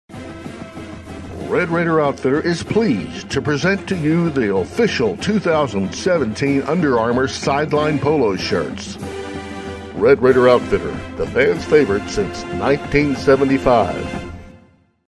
2017 RRO TV SIDELINE POLOS
Designed for the Sideline and for YOU, the 2017 Texas Tech Under Armour Sideline Polo is here!